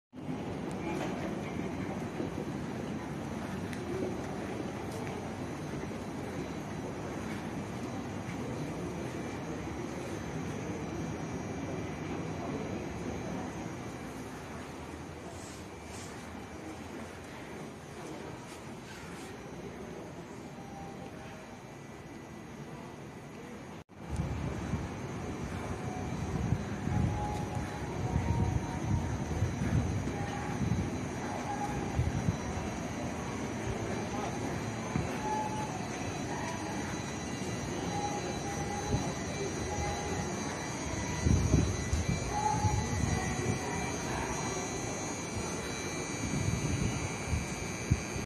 京王7000系動物ラッピング車両
京王線の7000系電車です。
高幡不動↔️多摩動物公園を走る7000系は、かわいい動物をラッピングした編成となります。
ピンクで非常に目立つ存在です。
車内も各車両で動物のラッピングとなり、子供は喜びます！
いった先も楽しいですが、移動も楽しくなります。

🚃小さい頃から特に鉄道が好きです。他の乗り物も好きですよ！！
🚃嫌いな乗り物は『体重計』デス。。。
🚃コメントもいただけると助かります。